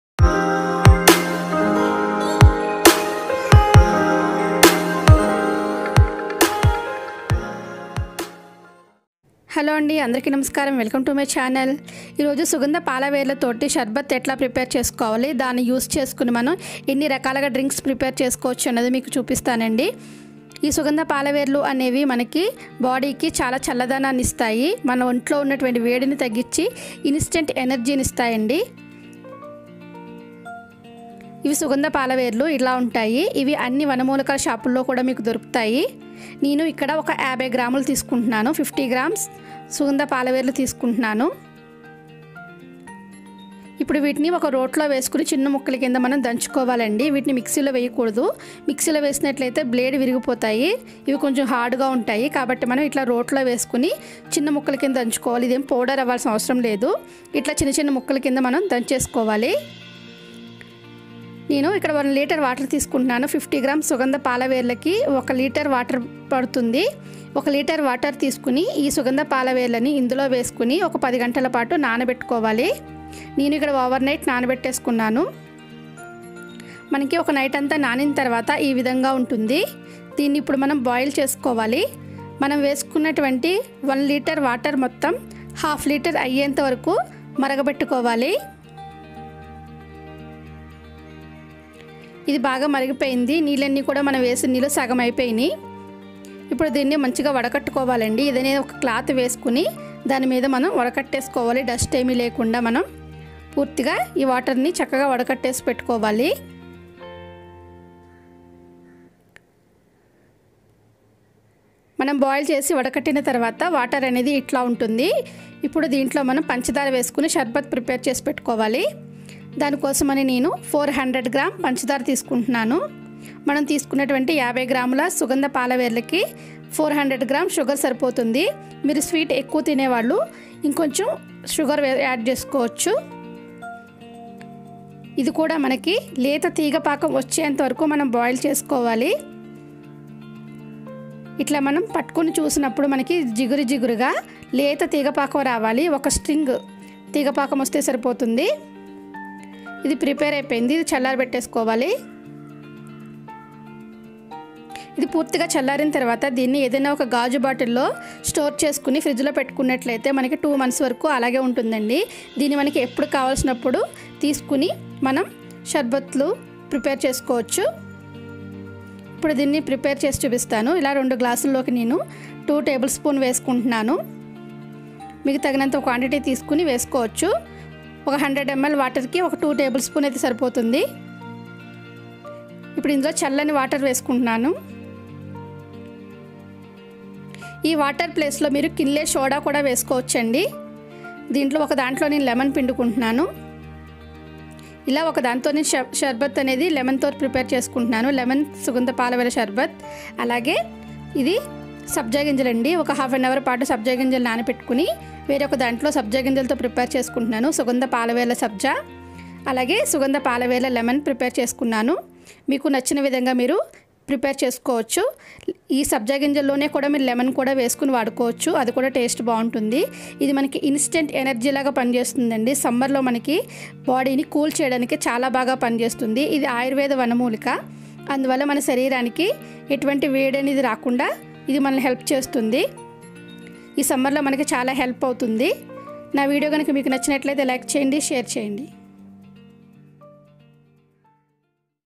సుగంధ పాల వేర్ల షర్బత్...నన్నారి షర్బత్.. Sugandha Veru (root) sharbat.Nannari sharbat..
satya's Kitchen & crafts..

Today I am going to share with you a recipe Sugandhi sharbat or nannari sharbat..

The best Summer Drink Sugandhi sharbat or nannari sharbat..

INGREDIENTS...
Sugandi root or nannari root 50 grm
 sugar 400 grm

sugandi sharbat or nannari sharbat it is a natural coolant, and it helps our body to Fight the scorching summer heat. Keeping our body hydrated is a must, and these light and refreshing sharbaths are great alternative to the non healthy aerated drinks.. it's helps to purify our blood, and also it increases the energy level..